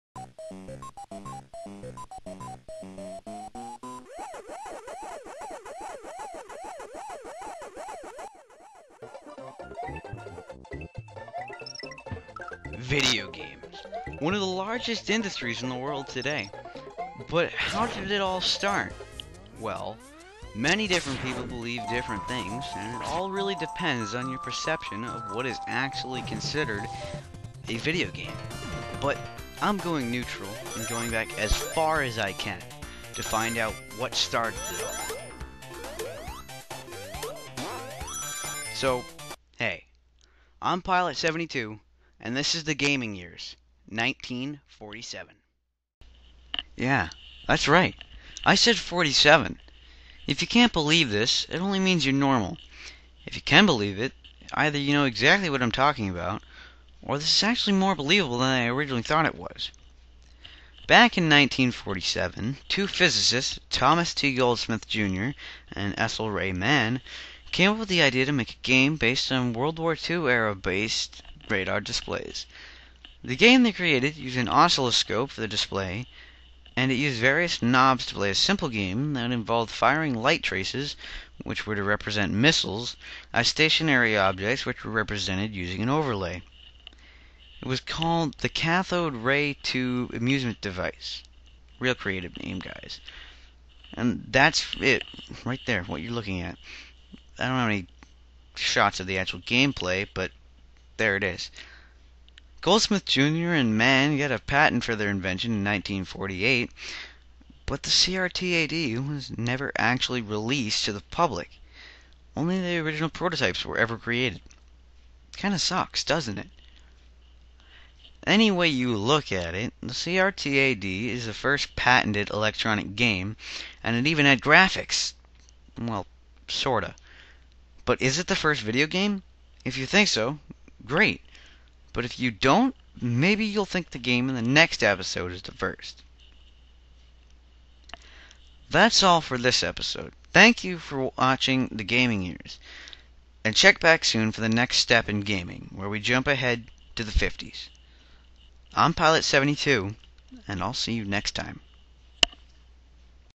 The Gaming Years: 1947
This is the first episode of a series of videos which will tell the history of video gaming, one year at a time. And we start with 1947.